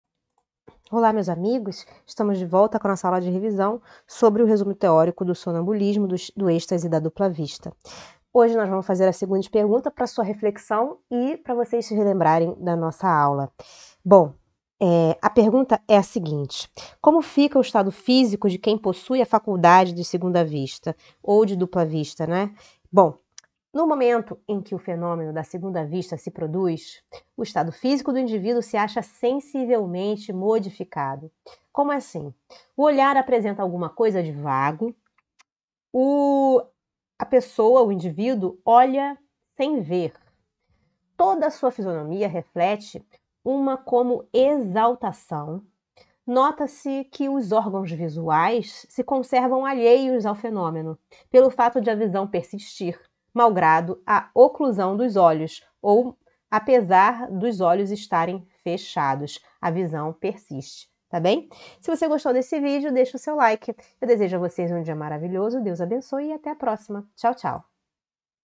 COMO FICA O ESTADO FÍSICO DE QUEM POSSUI A FACULDADE DE SEGUNDA VISTA OU VISTA DUPLA?
AULA DE REVISÃO

CAPÍTULO 8 - Da emancipação da alma

• Resumo teórico do sonambulismo, do êxtase e da dupla vista